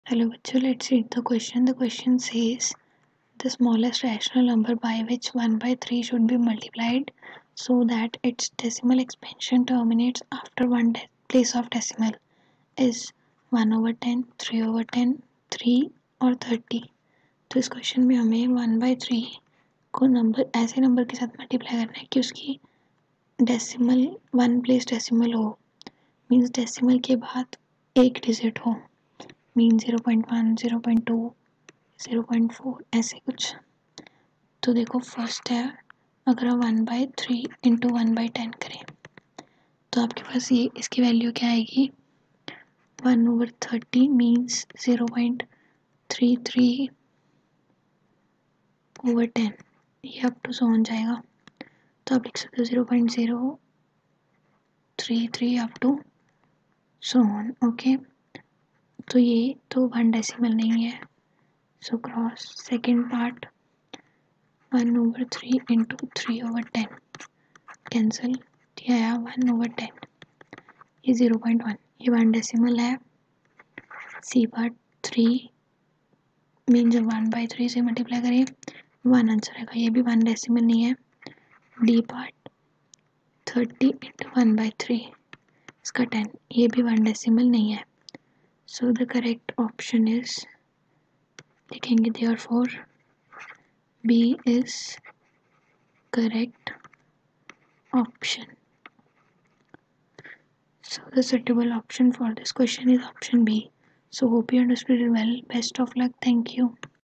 The smallest rational number by which 1/3 should be multiplied so that its decimal expansion term...
The smallest rational number by which 1/3 should be multiplied so that its decimal expansion terminates after one place of decimal, is
(a) 1/10
(b) 3/10